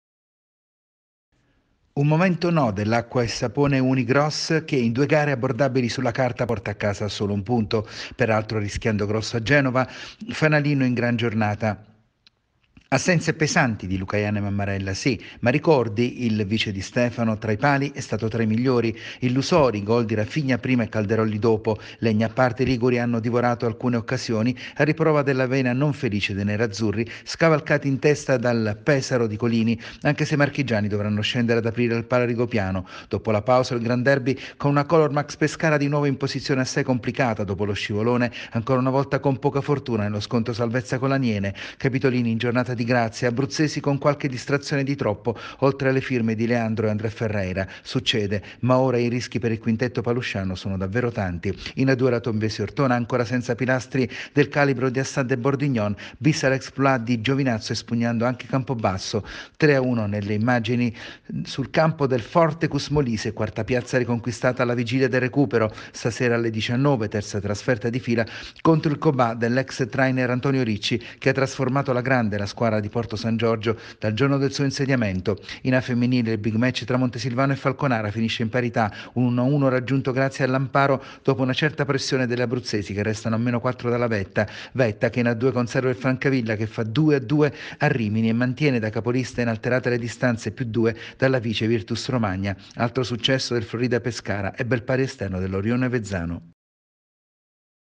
L’Acqua & Sapone Unigross perde la vetta. Bene la Tombesi
Futsal. Brutto weekend per le abruzzesi di A1. L’Acqua & Sapone Unigross perde la vetta mentre la Colormax va ko in casa. In A2 sorride ancora la Tombesi che supera a domicilio il Cus Molise. Questa sera la squadra ortonese tornerà in campo alle 19:00 a Fermo per il recupero contro il Futsal Cobà, società colpita nelle ultime ore da un lutto per la scomparsa dei suoceri dell’allenatore abruzzese Antonio Ricci, ex Montesilvano e Acqua & Sapone